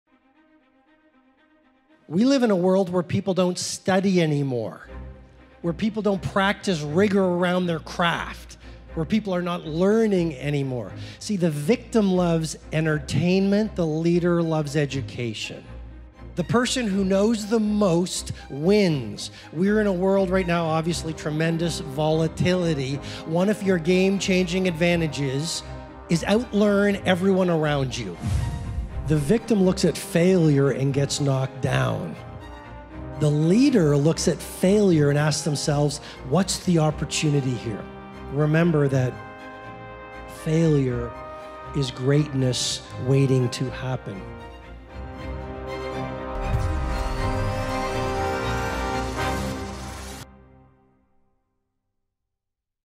Session Highlights: The Anatomy of Future Leader
Robin Sharma, Author and Leadership Expert, discusses the assets and anatomy of future leaders at the World Government Summit 2017.